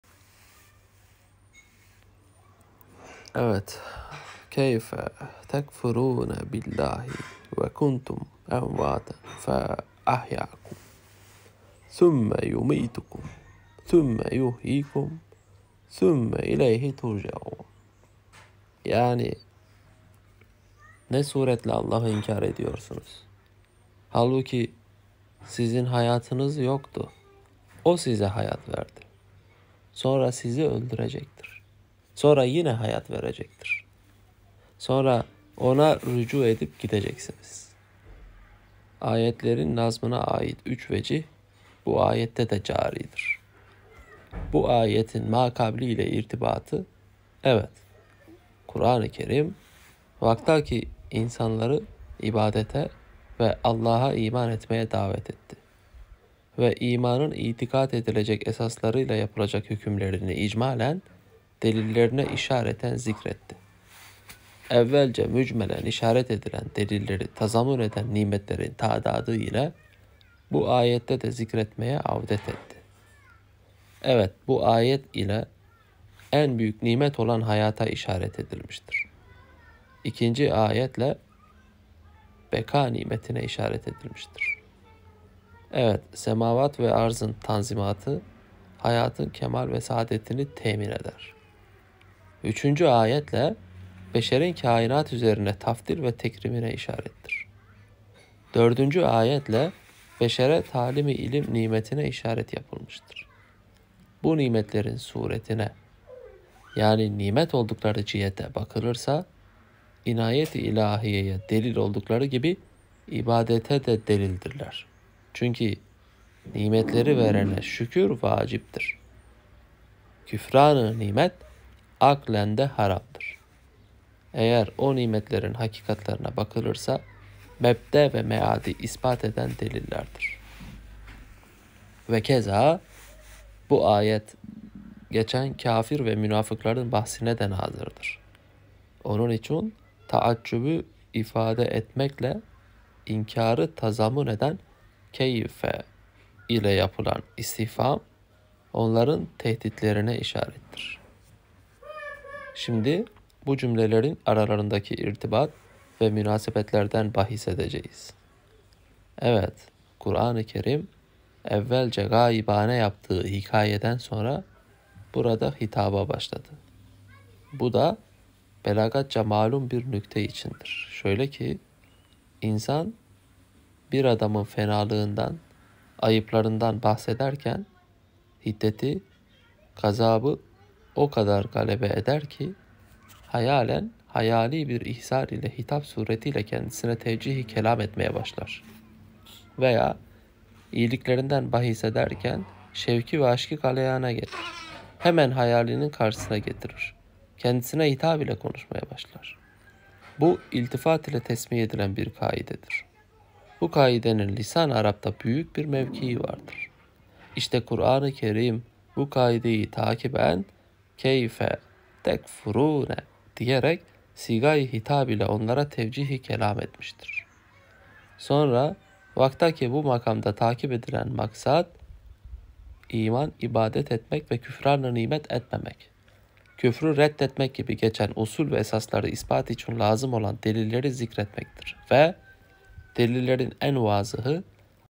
“Ne Suretle Allahı İnkâr Ediyorsunuz ? / Halbuki Sizin Hayatınız Yoktu O Size Hayatı Verdi.” / 1
"En büyük bir nimet olan vücudu, bu vücudumda büyütmek ve çoğaltmak için hayatı verdi."
Şualar - 67

Cümlesi ile

“Evet hayat, kudret-i ezeliyenin en büyük ve en ince ve en acib bir mu'cizesidir ve bütün nimetlerden üstündür”
İşârât-ül İ'caz - 178

Cümleleri arasın da bir çelişki yoktur. Zira Vücudun yani varlık sahasının en büyük bir nimet olduğunu anlamak ve idrak edebilmek için Hayat nimetinin olması mutlak şarttır ve hayatsız bir vücudun varlığı asla hissedilmez , anlaşılmaz.